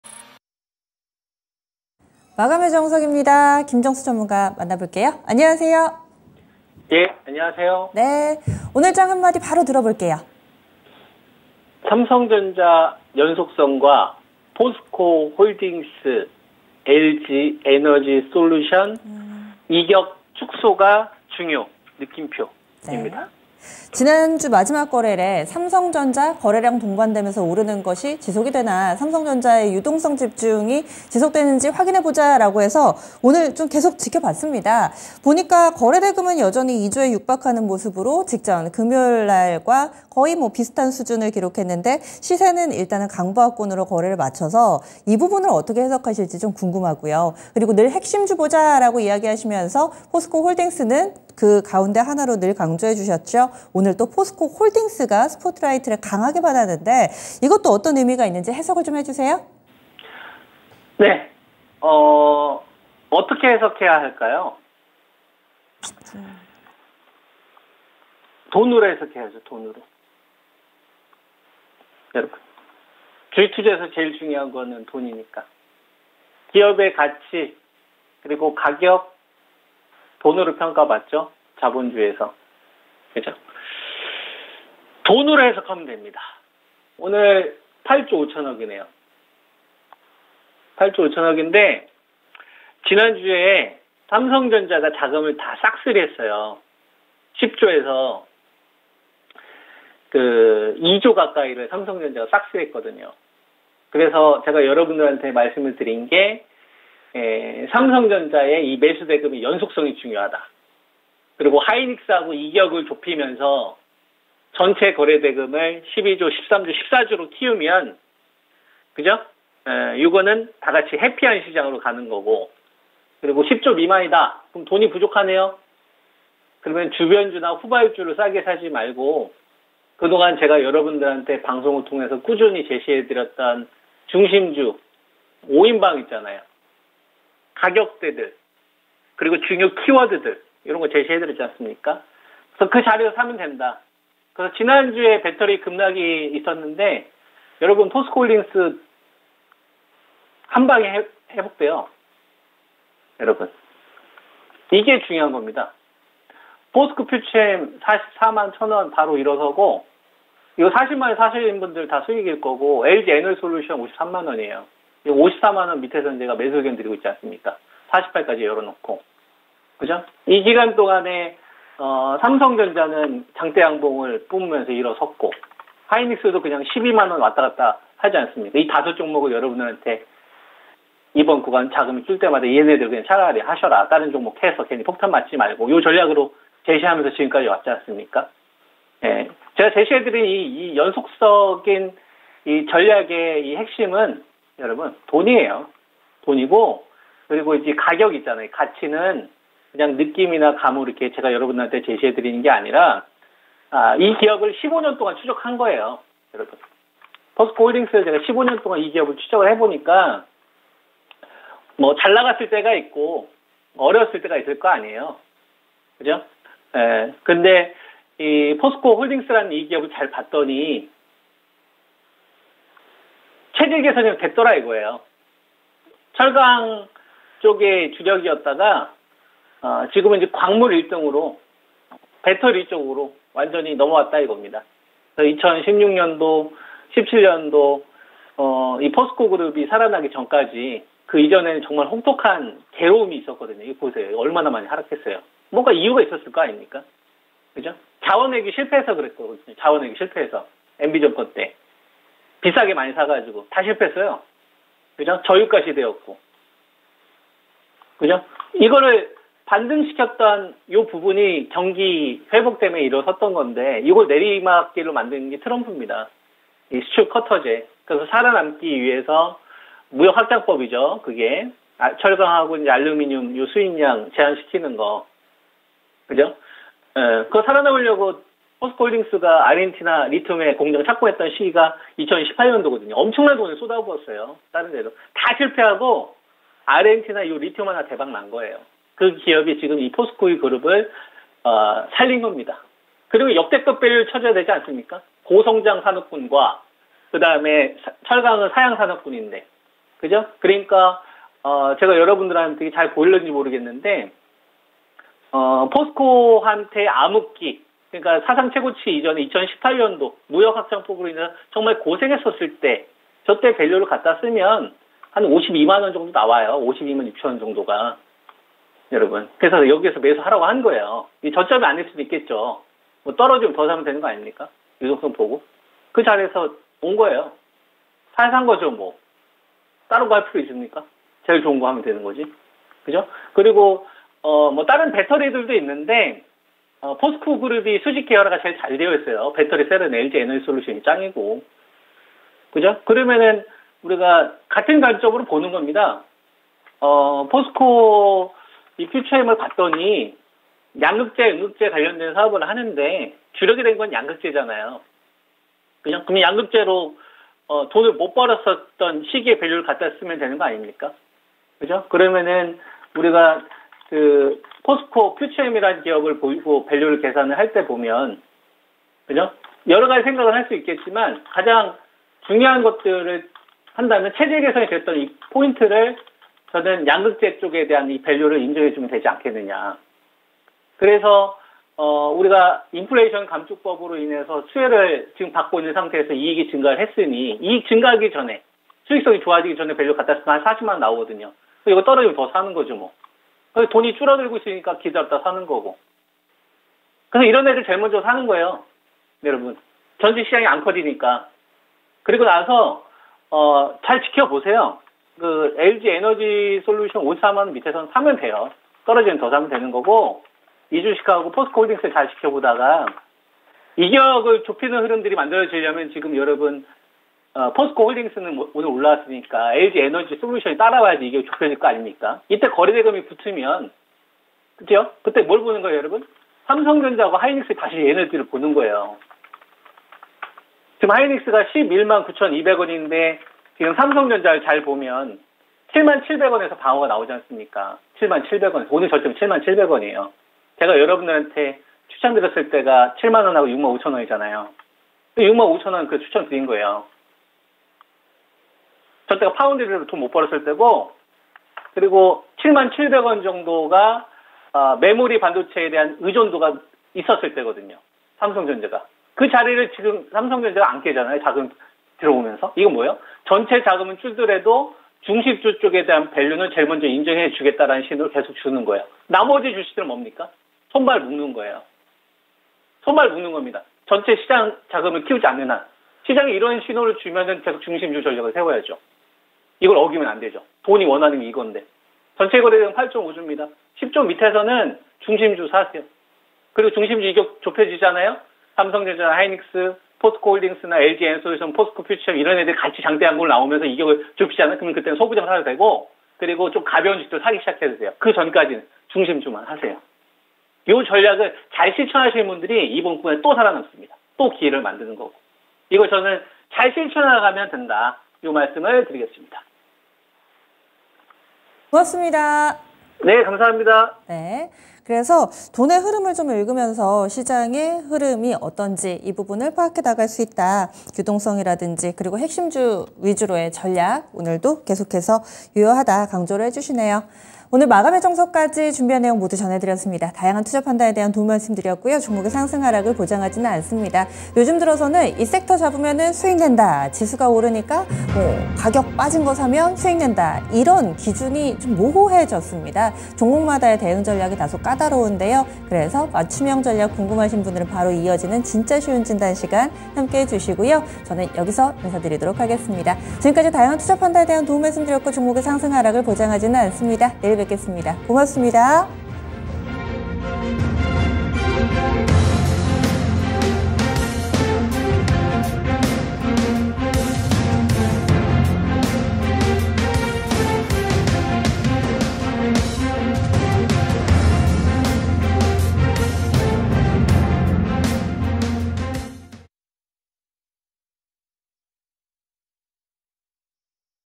포스코 예견된 수익과 앞으로의 시장 대비 전략 [마감의 정석]
김정수의 마감의 정석
진행 : 박상정
김정수(토마토투자클럽)
한마디 : ' 삼성전자 ' 연속성과 ' POSCO홀딩스/LG에너지솔루션 ' 이격축소가 중요!
- 삼성전자 매수 대금의 연속성 중요, SK하이닉스와 이격을 좁히면서 전체 거래 대금을 14조까지 키우면 모두가 해피한 시장
- 10조미만으로 떨어진다면 주변주/후발주를 싸게 사지 말고 꾸준히 제시했던 중심주 5인방 기다리던 가격대에서 사면 된다
- POSCO홀딩스, 금일 한방에 회복
- 포스코퓨처엠, 40만원에 매수했다면 다 수익
- LG에너지솔루션, 54만원 이하에선 1차 매수
- 삼성전자/SK하이닉스 견고한 모습
- POSCO홀딩스, 철강이 주력이었다가 지금은 배터리쪽으로 넘어왔음
 - 18년도, 고생했었을때 밸류는 52만원, 지금 그자리에 왔고 떨어지면 더 사면된다
- 포스코퓨처엠, 이익 증가하기전 밸류 40만원...여기서 떨어지면 더 사면 된다
- LG에너지솔루션, 이격을 좁히면서 거래 대금이 붙는다면 삼성전자/SK하이닉스를 보자
- 삼성전자, 70700원 방어나오는 모습(메모리 반도체 의존했던 밸류), 6.5만원때 추천했음
 - 전체 자금은 줄더라도 중심주 밸류는 인정해주겠다는 의미
- 10조 밑에선 중심주를 사고 중심주들의 이격이 좁혀진다면 그때 소부장/가벼운 주식들 접근 가능

전략 : 한정된 매수자금 이기에 배터리/반도체/자동차 중심주 부터 매수.

▷김정수 전문가

* KP2,480 ~ 2,580까지 10조의 자금으로 반등나온 시장
전체자금이 14~15조 증가하지 않으면서 반등나오는 시장일수록 더 중심주로!
ex)삼성전자,SK하이닉스,한미반도체,POSCO홀딩스,LG에너지솔루션,포스코퓨처엠

* KP 2,550에서 2,650까지 갈때 삼성전자 매수대금이 2조, 3조 연속적으로 증가하고
SK하이닉스와 이격을 좁히며 전체자금을 14조 , 15조시장으로 키울때는 소부장도 매수

* KP 2550 이상에서 삼성전자의 매수대금이 연속적이지 않고 이격도 정체된다면
세달간 이어온, 미래차 중심주를 유동성 보면서 매집! 분할매수 하자!
만약 , 삼성전자/SK하이닉스와 POSCO홀딩스/LG에너지솔루션이 이격이 함께
좁혀지면서 전체 매수자금이 증가하는 구간은 배터리 중형주도 사야한다.